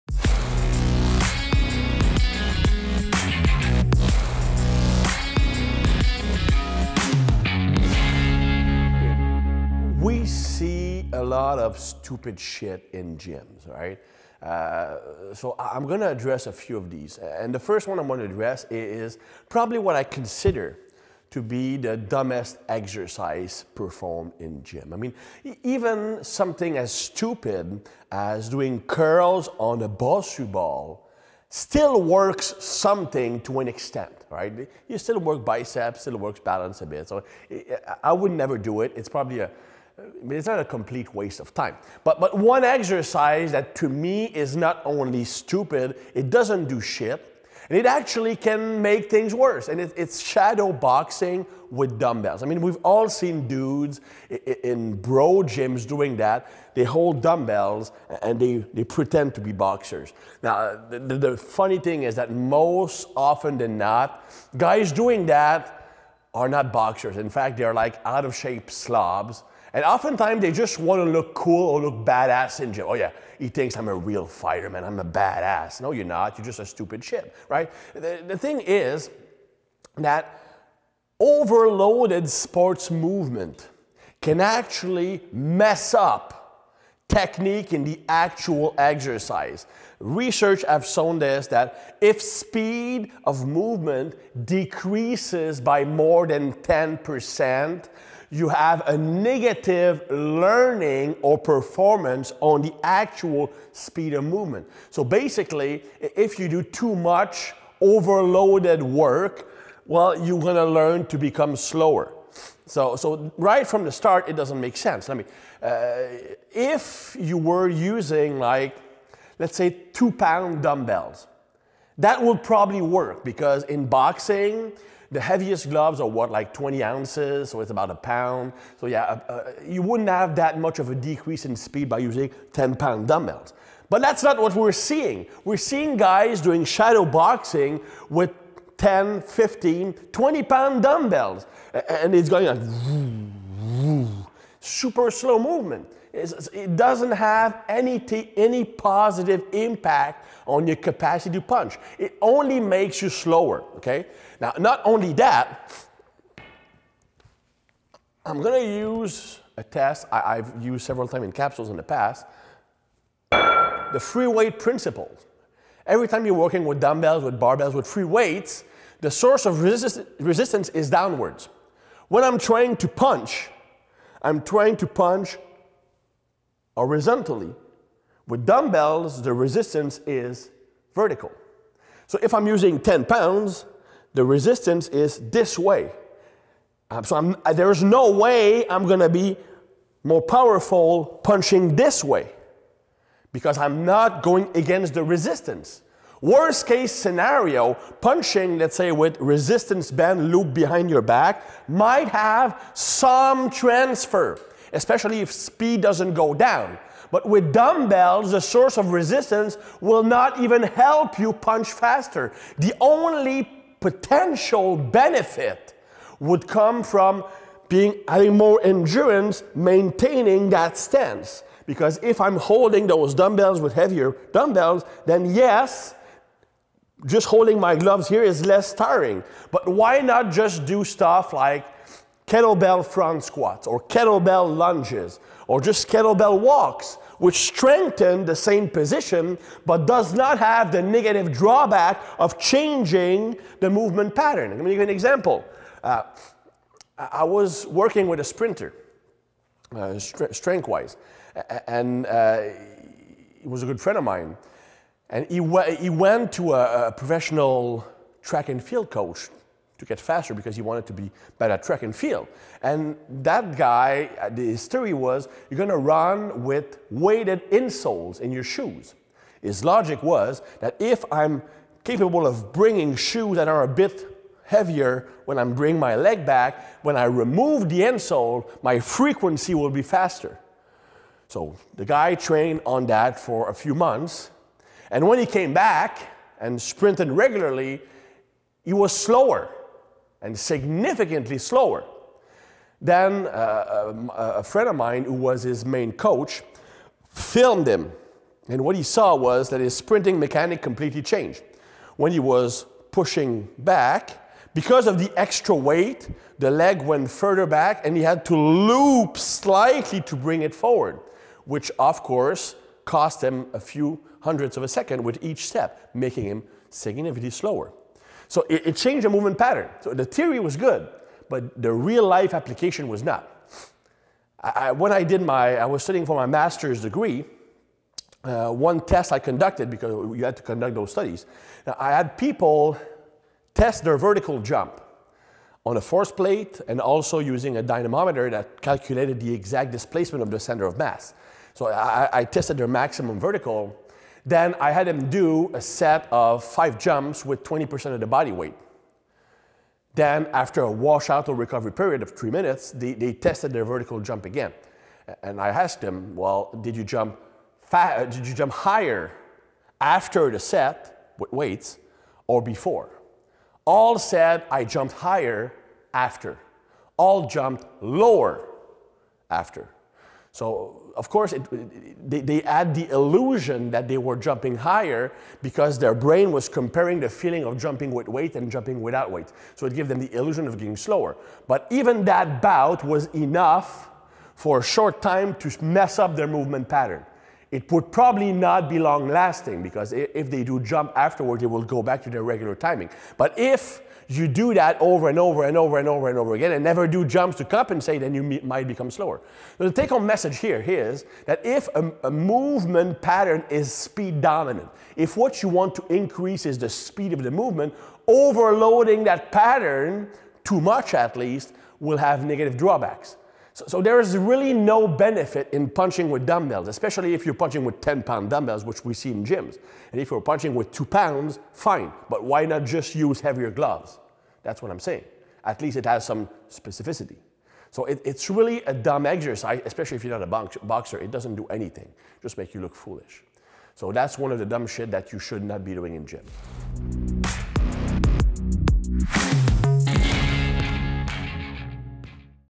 THINGS THAT PISS ME OFF - SHADOW BOXING WITH DUMBBELLS
We see a lot of stupid things in gyms. In this video series, Chris will address a few of these.  In this video, he discusses what he considers to be the most useless exercise still being done in gyms: shadowboxing with dumbbells.

Thibarmy - @thibarmy